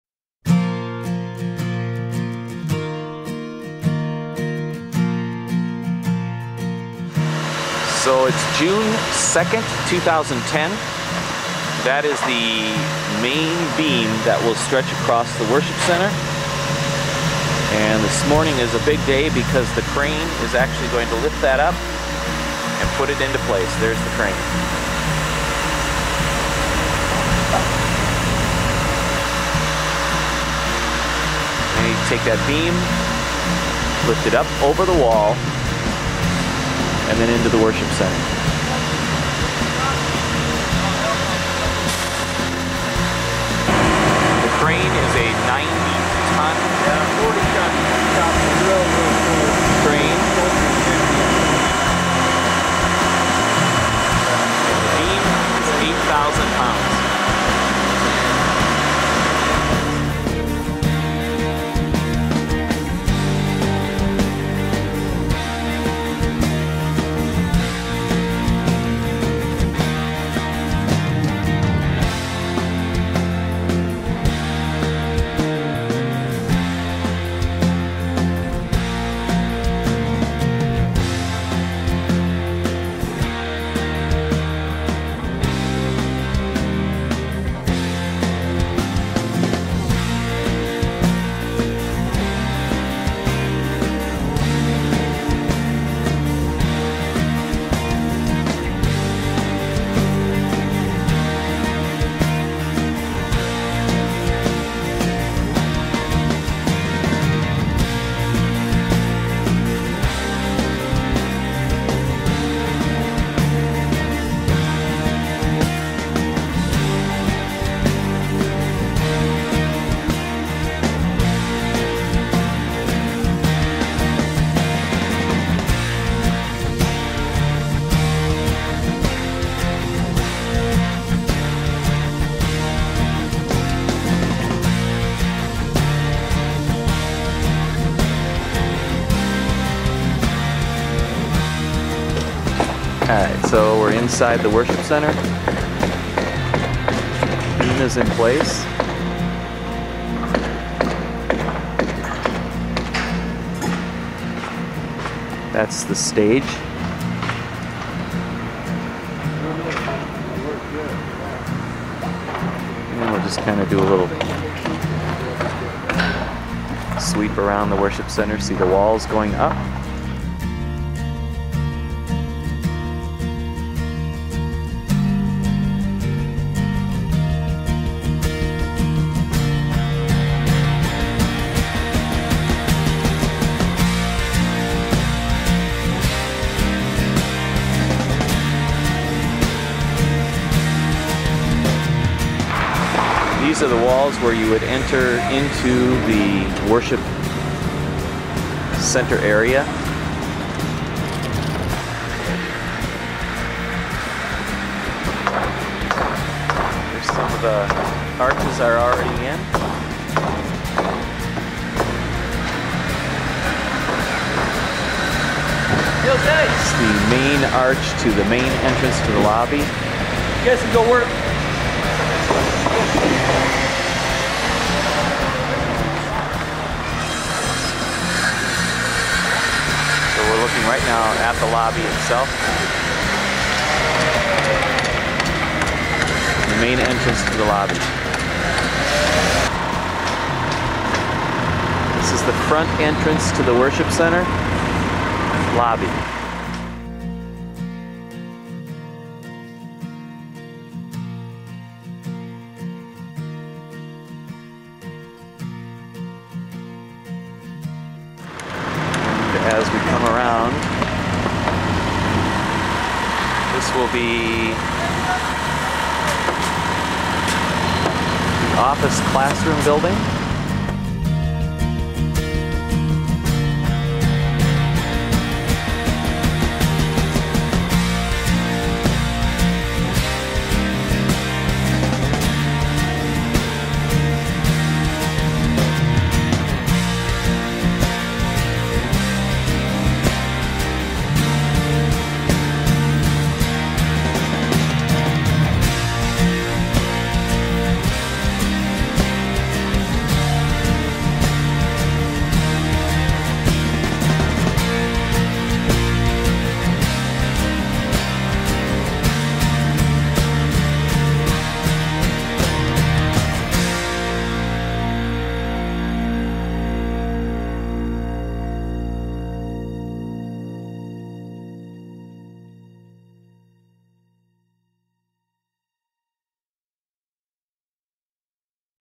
Community of Grace Building Update04
Community of Grace Lutheran Church in Peoria, AZ building project update June 2, 2010. The main beam for the worship center roof gets put in place!!!